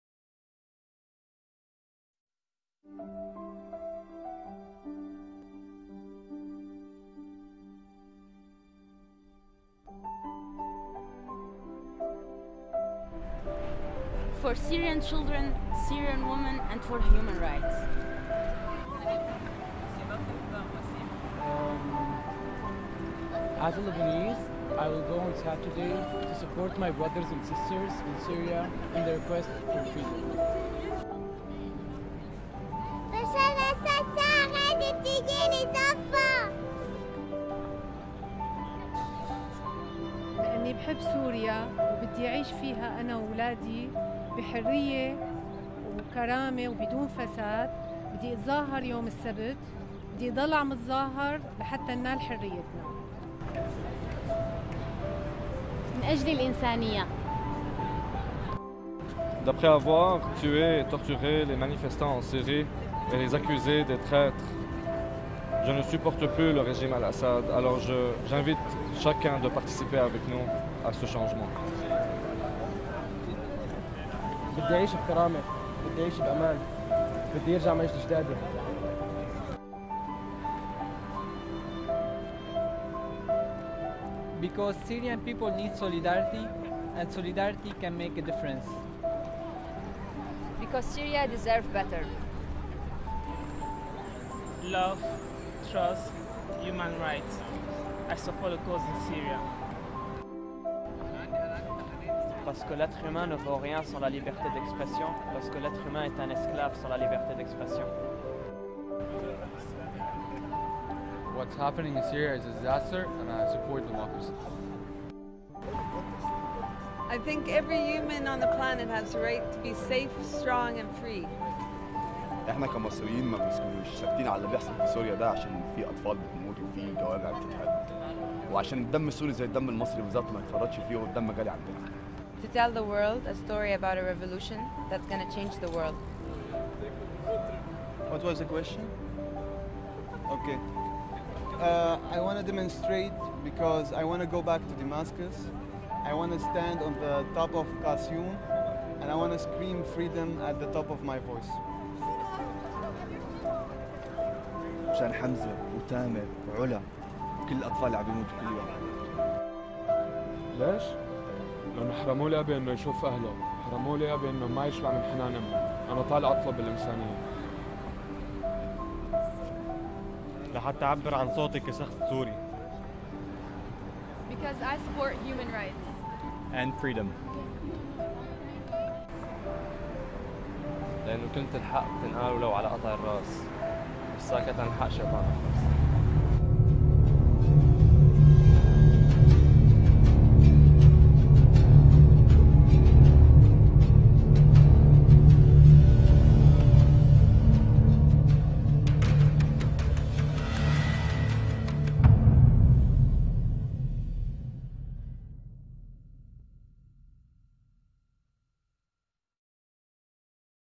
Long life world solidarity - Montreal in Canada for the Syrian people 24.10.2011
OCT 29 _ March for Syria's Freedom _ Montreal - Official Promo - YouTube
Long life world solidarity against the oppression of the sadistic dictator Bashar Al-Assad -  From Canada for the Syrian people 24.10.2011